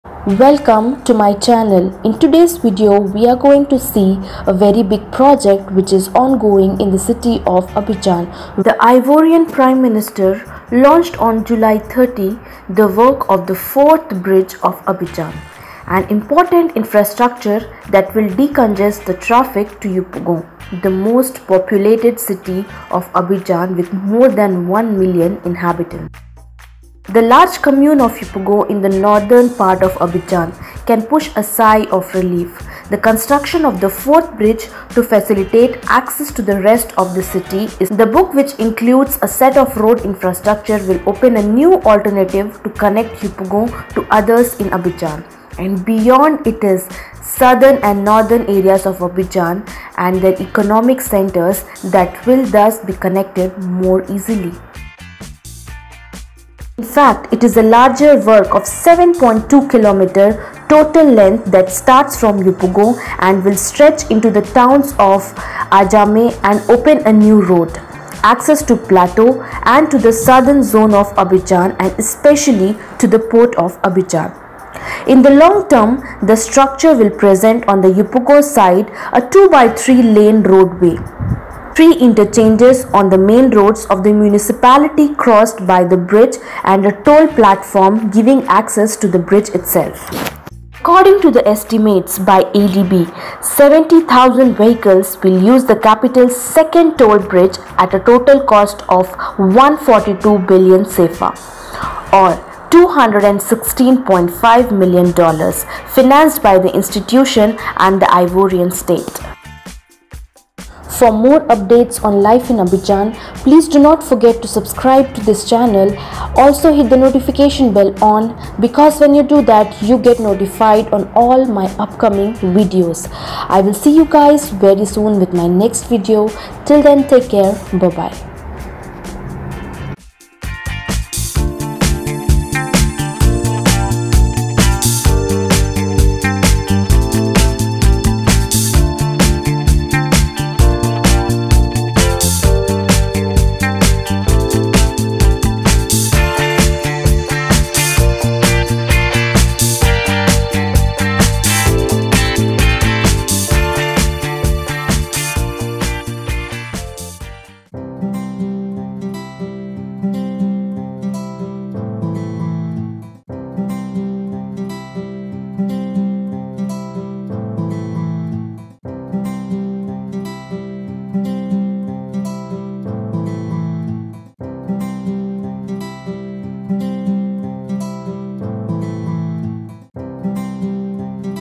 4ème pont d’Abidjan Yopougon-Plateau | 4th Bridge in Abidjan | Brown and Strong
4ème pont d’Abidjan Yopougon-Plateau | 4th Bridge in Abidjan | Brown and Strong
The construction of the bridge connecting the commune of Yopougon to Plateau called “4th. Pont d'Abidjan ”in order to improve traffic flow and provide an alternative to motorway users between Yopougon, Attécoubé and Adjamé.Scheduled for a period of 30 months, the work on the fourth Abidjan bridge is costing CFAF 142 billion .The project is financed by the African Development Bank (ADB) and the State of Côte d'Ivoire.

For all the new updates